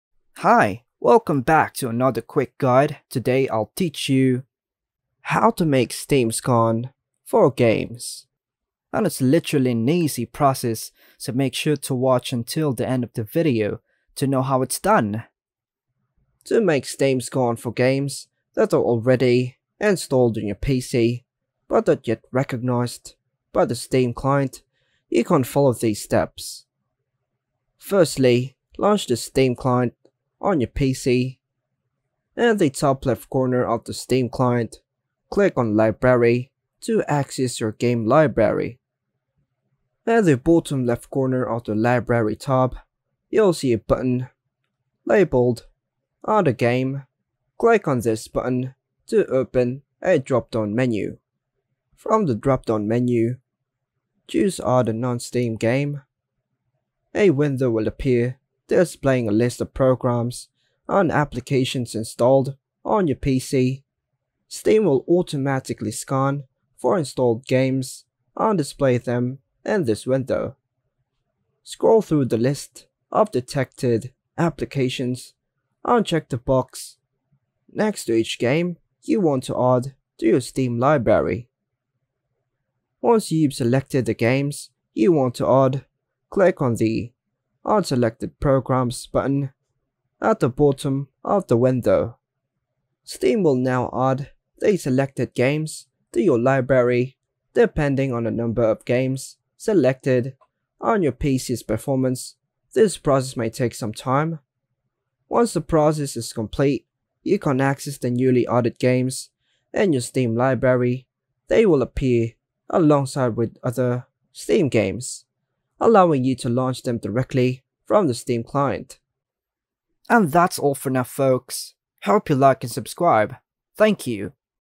How To Make Steam Scan For Games (Quick & Easy Method)
In this video, you will learn how to make Steam scan for games. Use this easy method to have Steam locate games that are already downloaded onto your PC.